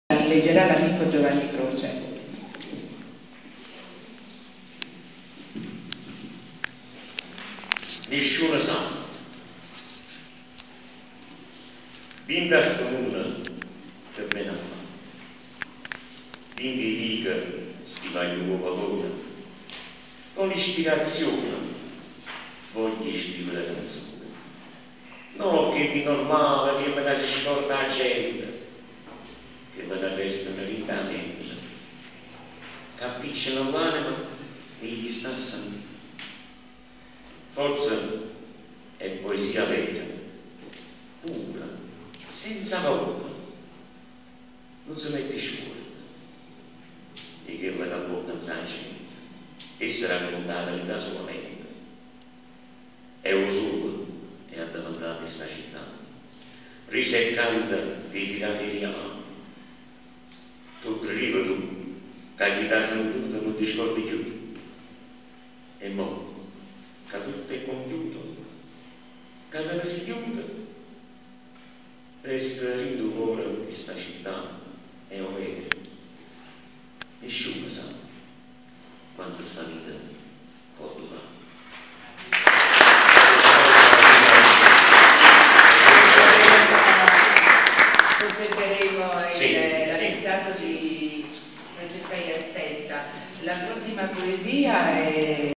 Giovanni Croce declama versi di Francesca Iazzetta "NISCIUN SA"
Video amatoriale  per una serata dedicata al grande amico Pino Daniele organizzata da Giovanni Croce e Clara Raffaele nella sala della Biblioteca Labriola di Napoli, quartiere di San Giovanni a Teduccio il 13 marzo del 2015